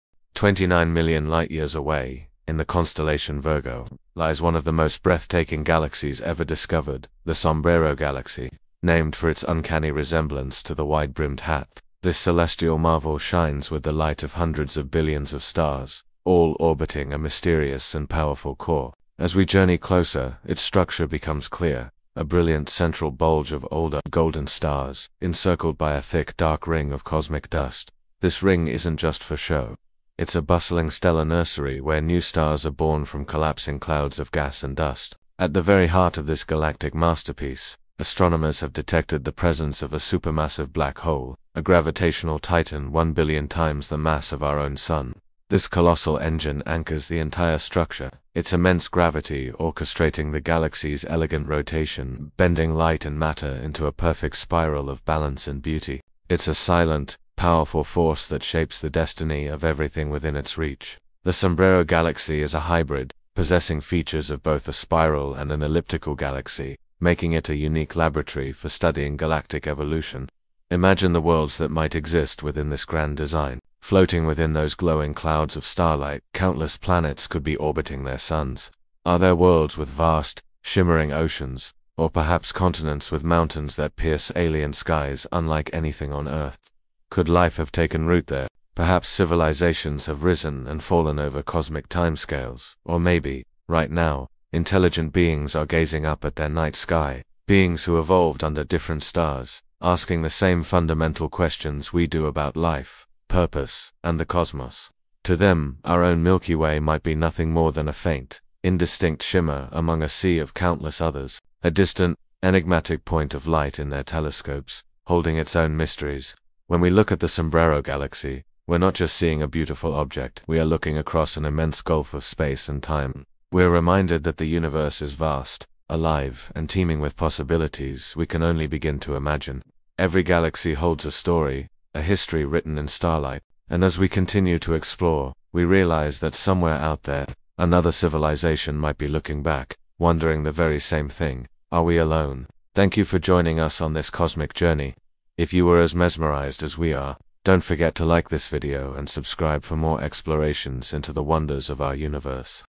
🪐 The Sombrero Galaxy: Could Life Exist Inside This Cosmic Hat of Light?
The Sombrero Galaxy, located 29 million light-years away, is one of the most mysterious galaxies in the universe. With a massive bright core and a ring of dust glowing like a celestial hat, it may hide billions of worlds — and possibly intelligent civilizations waiting to be discovered.